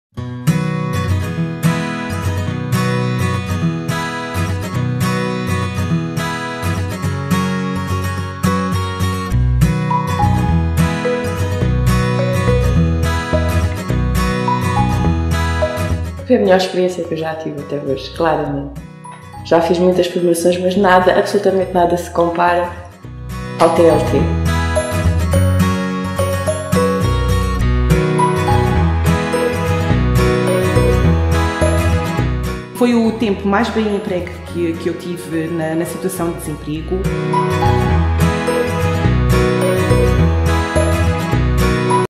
2.ª edição do Programa de Capacitação Talentos em Livre Trânsito
2.ª edição do Programa de Capacitação Talentos em Livre Trânsito, promovido pela Fundação Eugénio de Almeida, em Março de 2016.

Esta iniciativa é integrada no Programa de Empreendedorismo da Área Social da Fundação Eugénio de Almeida e tem como objetivo promover o desenvolvimento de competências pessoais e de capacitação para a empregabilidade e para o empreendedorismo.

A responsabilidade técnica e formativa é a da SAPANA.ORG, organização sem fins lucrativos.